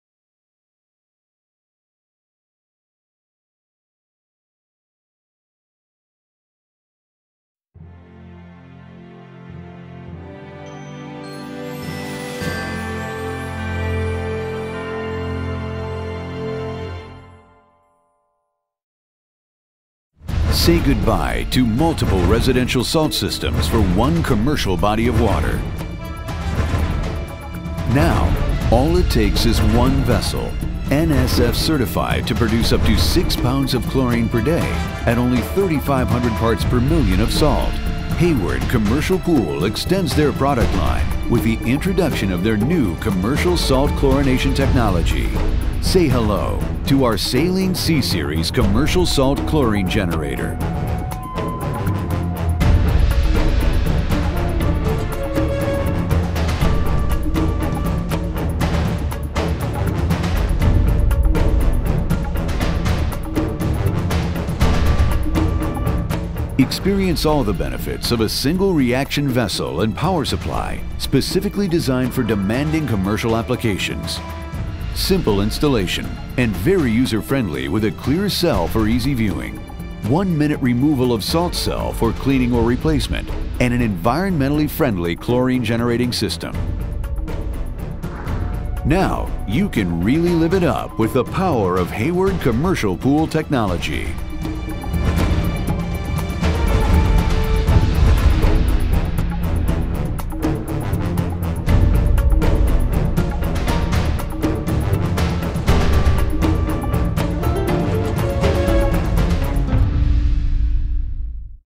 Hayward Commercial Salt Chlorine Generator Saline C 6.0
Hayward Commercial Pool is ready to unveil Saline C 6.0 commercial pool salt chlorine generator. This new eco-friendly, single vessel, low cost chlorine solution produces up to 6 pounds of chlorine per day. Key features include: commercial sizing 60Kgal outdoor, 90K indoor; compact design, single cell and power supply with easy install; industrial-grade, clear vessel and easy inspection; reverse polarity self-cleaning technology; plug n' play chemical automation - works great with our CAT Controllers; simple 1 minute electrode removal; eliminates transportation, handling and storage of hazardous chlorine; reduces chloramines and associated odors/corrosion; energy efficient power supply draws just 4 to 6 amps; 40% to 60% lower cost per lb of chlorine vs. sodium and calcium hypochlorite; game-changing initial investment and cost of ownership; simple, low cost installation vs. multiple cell alternatives. Now available.